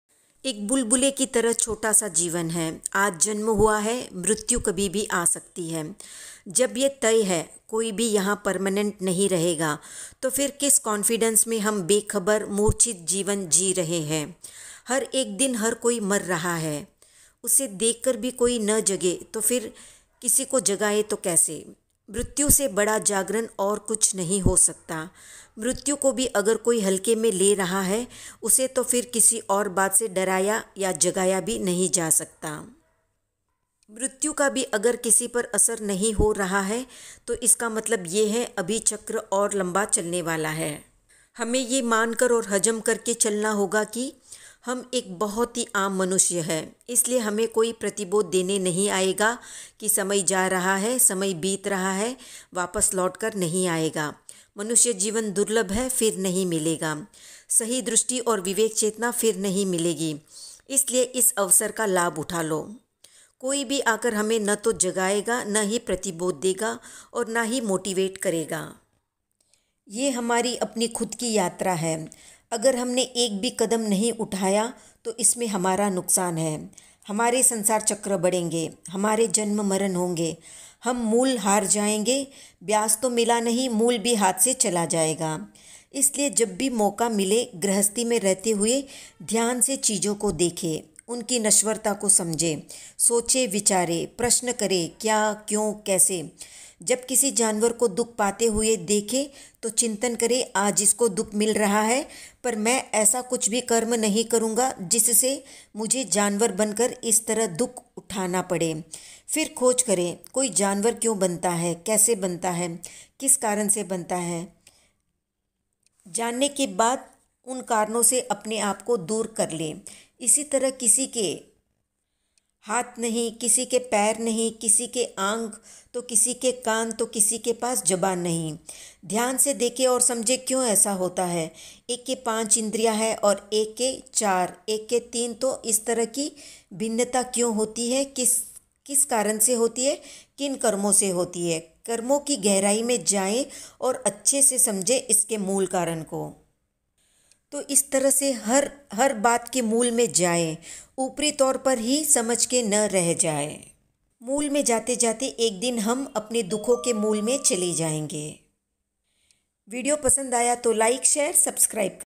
ऐसे जाए मुक्ति के नजदीक
महावीर कहते है ऐसे लोग जाते है नरक में
these kind of people goes to hell

, भगवान महावीर
नरक में क्यू गए ???